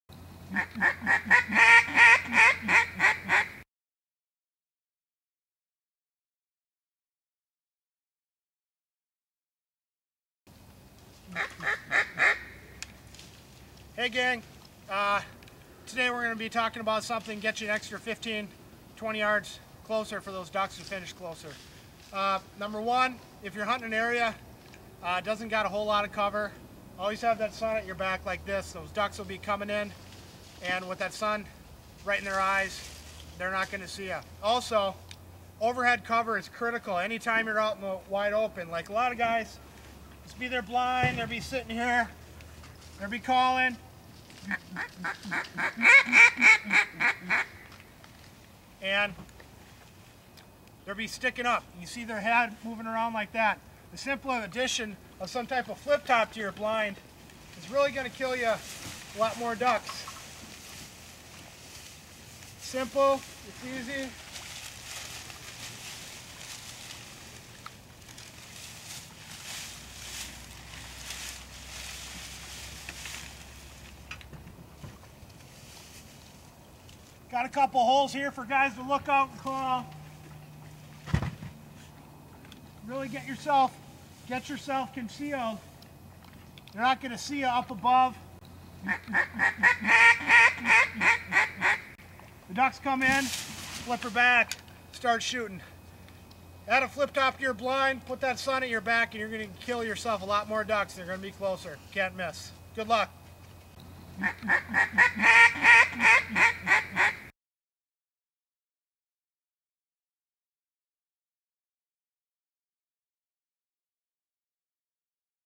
Duck Hunting Boat Blind Over Head Cover Tips
Overhead cover on your duck boat blind or goose blind will increase your success. Pressured ducks and geese will circle many times looking for movement. The point of view overhead ducks and geese have is often not blinded by hunters. Do not overlook this critical area when building your boat blind or duck blind.

Adding overhead cover to your blind is simple and one of the most overlooked but most important things to conceal. If ducks see your head moving around, they will not finish close if at all.

Duck boats blinds are used by the staff at:

Performance Calls LLC:
Lakeville, MN